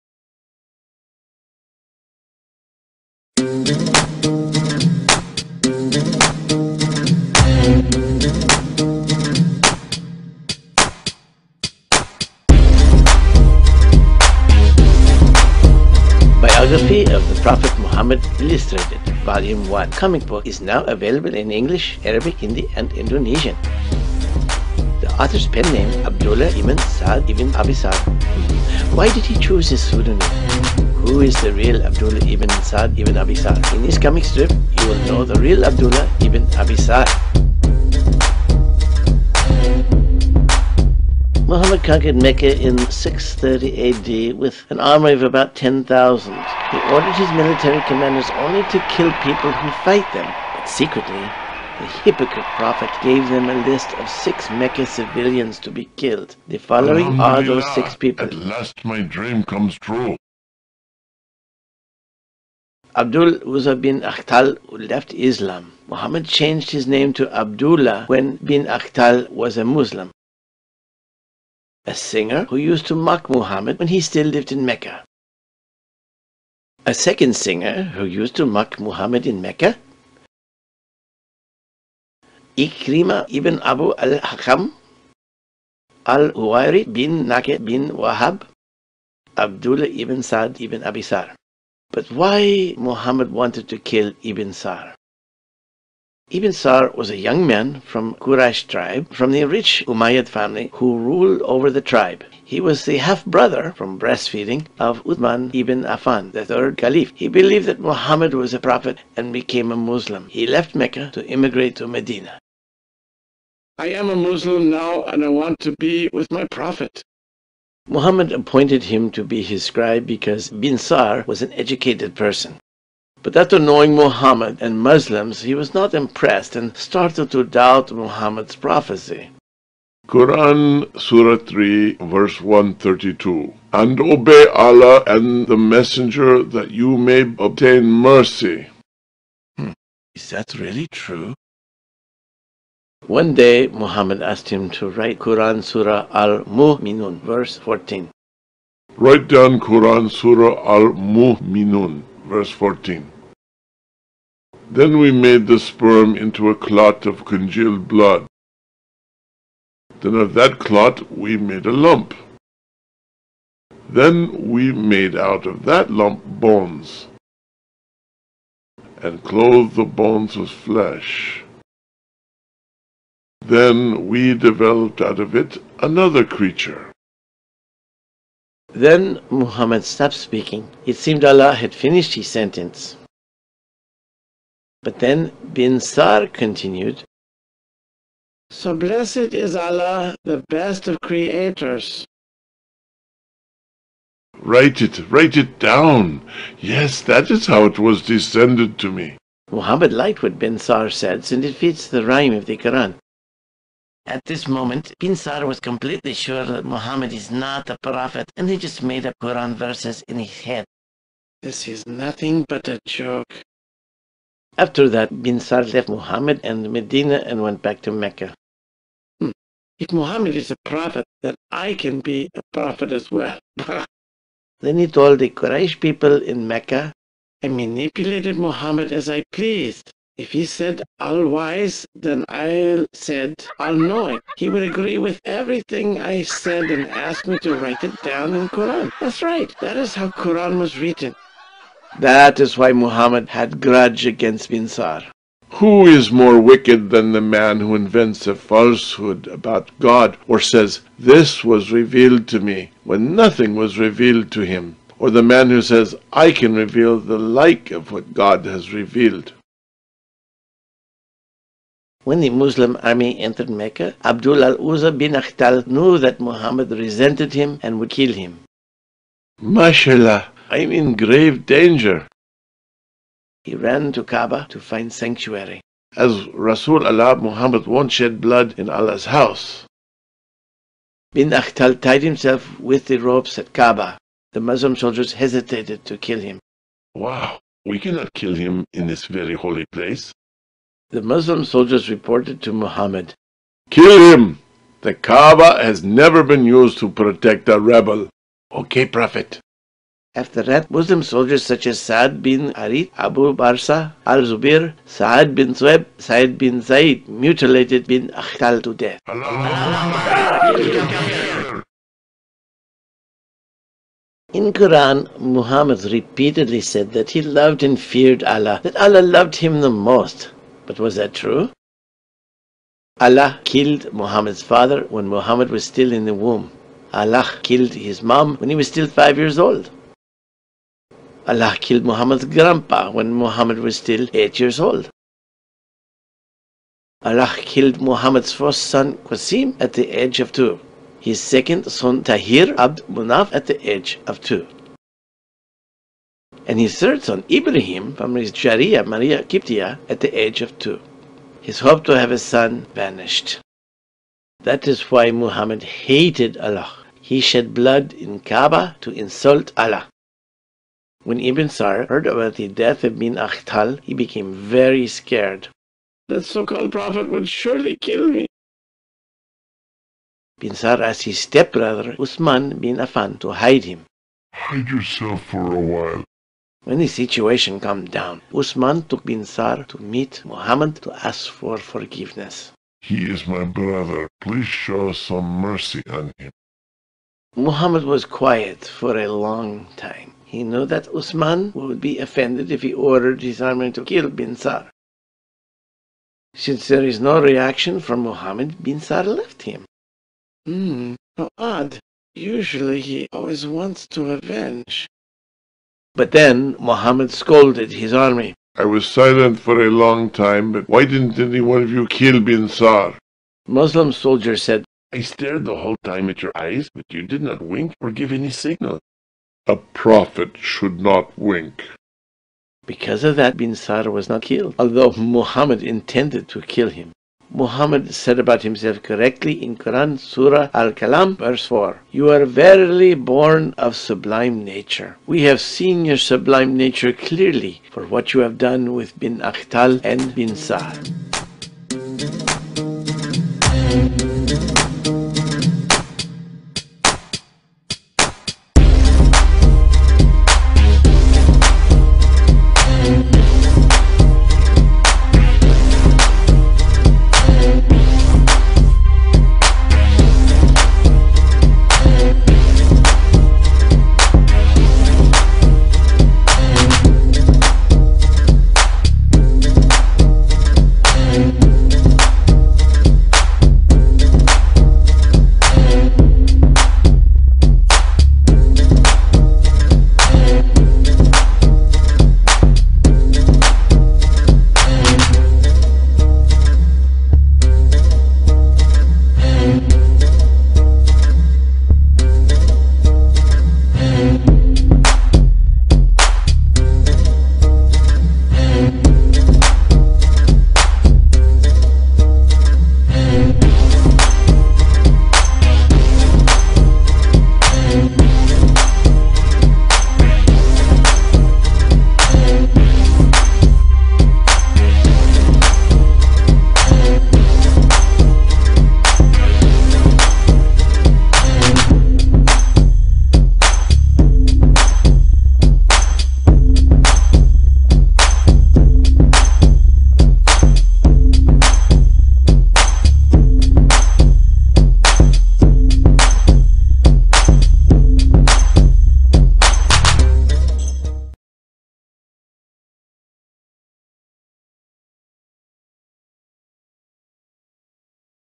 Who wrote the Quran Abdullah Ibn Sa'd Ibn Abi Sarh
Christian Prince